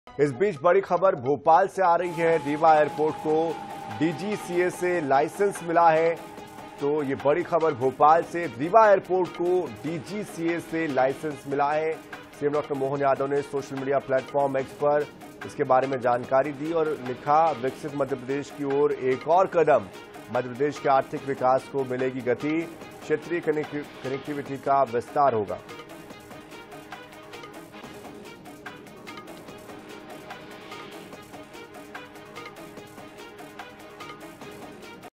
Rewa Airport को DGCA से मिला लाइसेंस | CM Mohan ने X पर पोस्ट कर कहा- विकसित MP की ओर एक और कदम
INH, Rewa Airport को DGCA से मिला लाइसेंस | CM Mohan ने X पर पोस्ट कर कहा- विकसित MP की ओर एक और कदम

Incoming Search Terms: